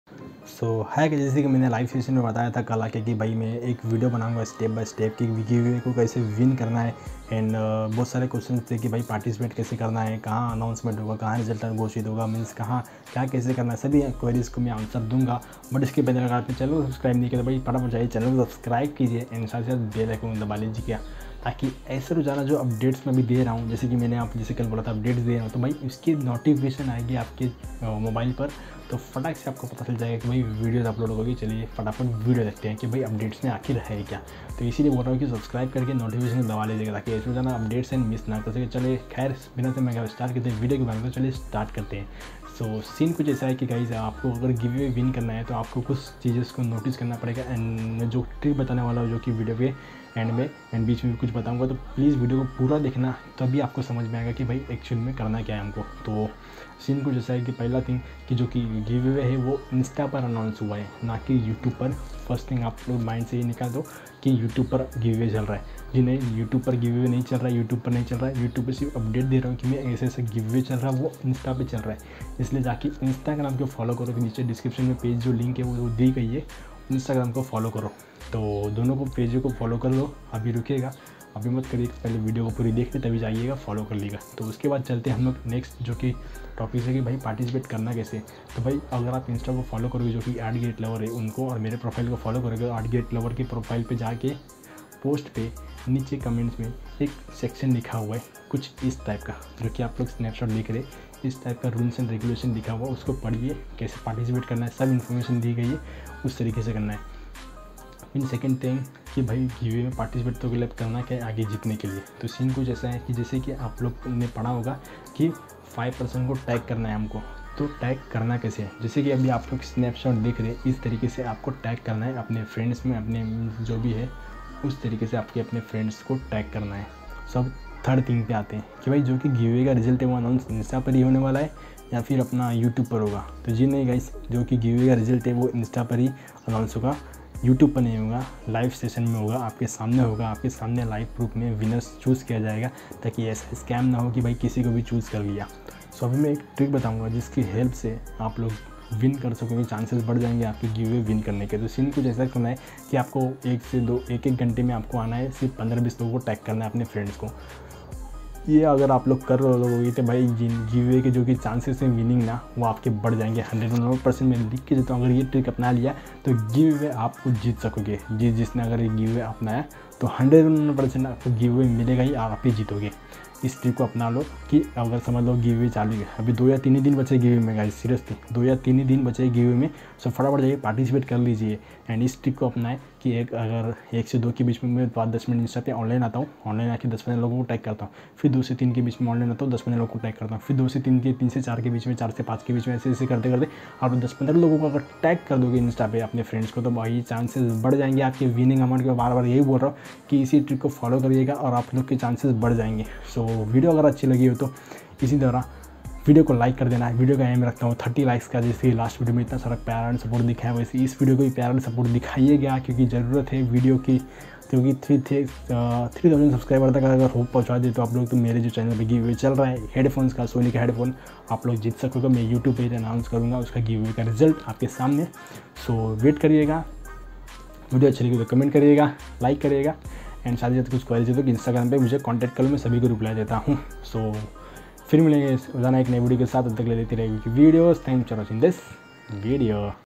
How To WIN This Giveaway 2021 || Avi k videos || Oneplus Nord CE 5g Free Giveaway 2021
Hi friends ☺️,

is video me Maine Aapko real fact bataya hai jiski help se aap asani se ek OnePlus Ka phone win kr sakoge voh bhi free of cost me
so, video ko Pura dekhna and acchi lage toh dosto k sath share krna .

page link